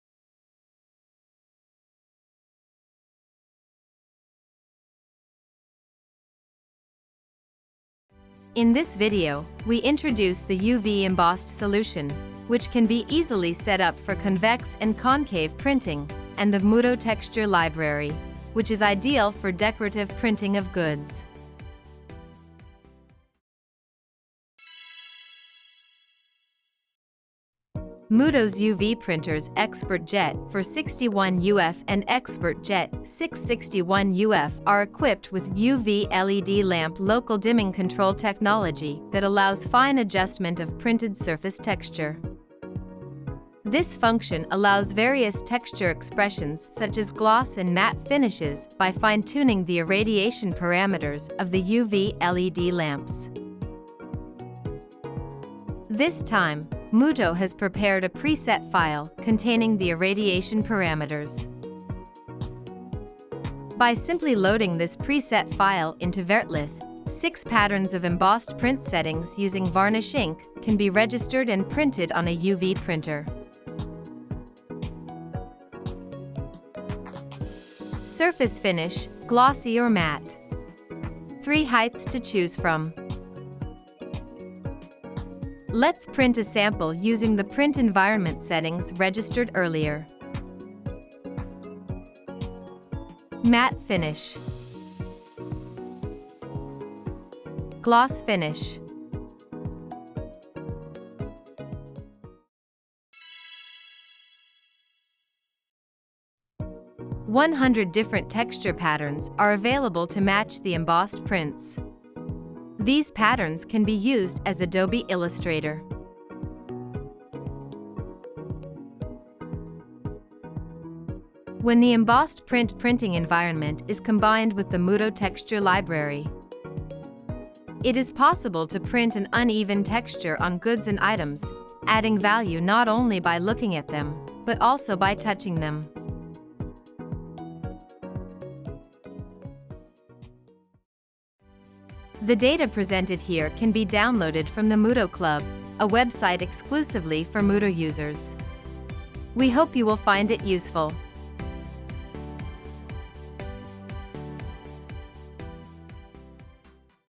Biblioteka struktur i wytłoczeń oprogramowania RIP Mutoh VerteLith do ploterów gadżetowych LED-UV.
Biblioteka ułatwia pracę z drukarkami do gadżetów LED-UV Mutoh XpertJet 461UF i 661UF, które powiększą możliwości zdobienia przy druku na różnego rodzaju gadżetach, co jest niezwykle istotne w szeroko pojętym biznesie reklamy, gdzie liczy się efekt i możliwości wykorzystania drukarki pod konkretne, spersonalizowane zamówienia. Dostęp do usługi uzyskują wszyscy nabywcy ploterów gadżetowych Mutoh, poprzez dedykowaną stronę internetową Mutoh Club. Więcej szczegółów można dowiedzieć się ze strony Atrium Centrum Ploterowe, jak i po obejrzeniu samego filmu.